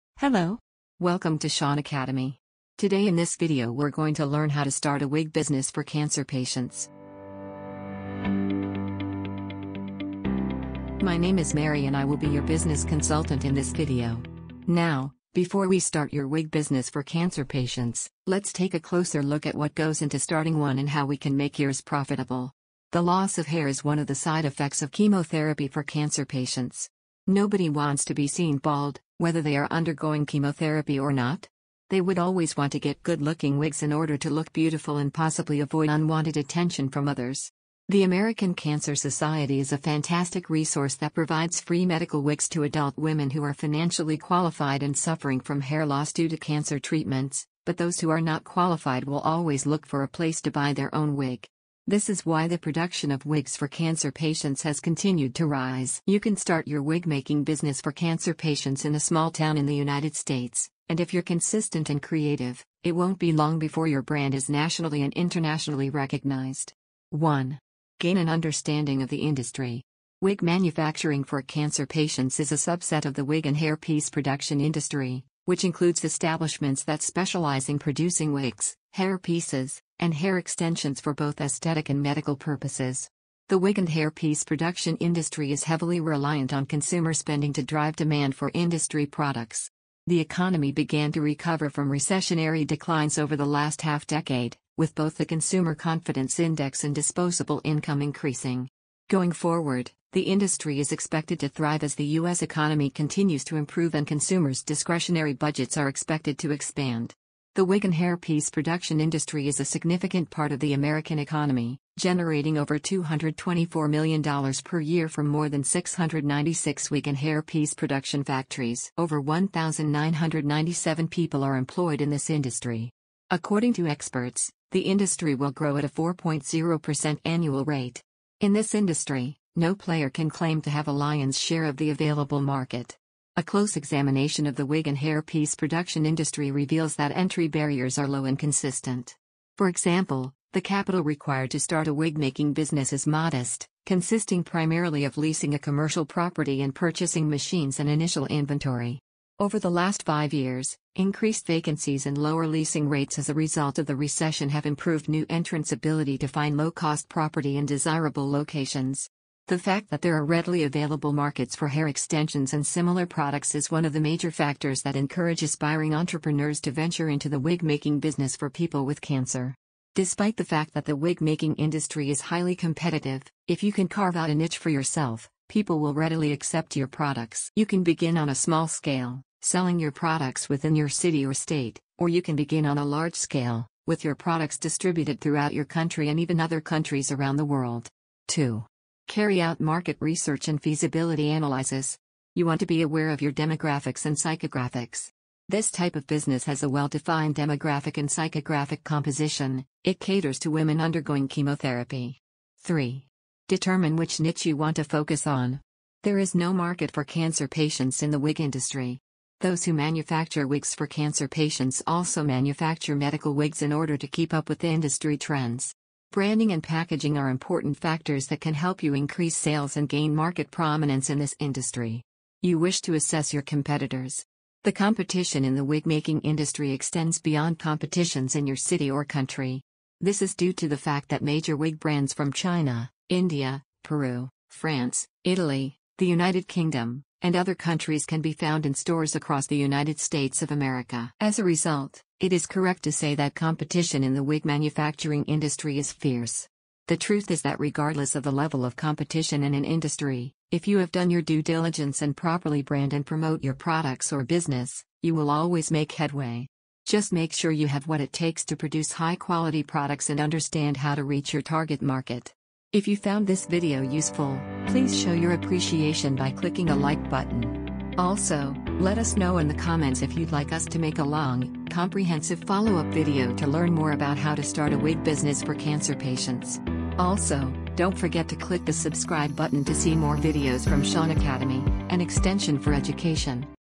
How to Start a Wig Business for Cancer Patients | Chemotherapy Wigs
In this video, you will learn how to start a wig business for cancer patients. Let's take a closer look at what goes into starting a wig business for cancer patients and how we can make yours profitable before you get started.

One of the side effects of chemotherapy for cancer patients is hair loss. Nobody wants to be seen as bald, whether or not they are undergoing chemotherapy.

They would always want to purchase attractive wigs in order to appear beautiful and possibly avoid unwanted attention from others. The American Cancer Society is a fantastic resource that provides free medical wigs to adult women who are financially qualified and experiencing hair loss as a result of cancer treatments, but those who are not qualified will always look for a place to buy their own wig.